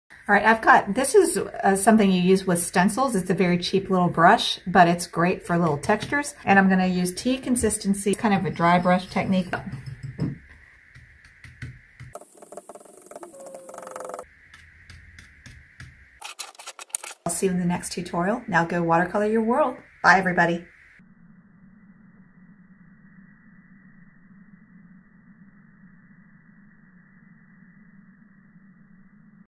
How to paint short fur with watercolor
I will explain a easy way you can paint short fur with a stencil brush - this works for cat fur, dog fur, any short fur you want to paint with watercolor.
to watch this and over 90 other paintings from start to finish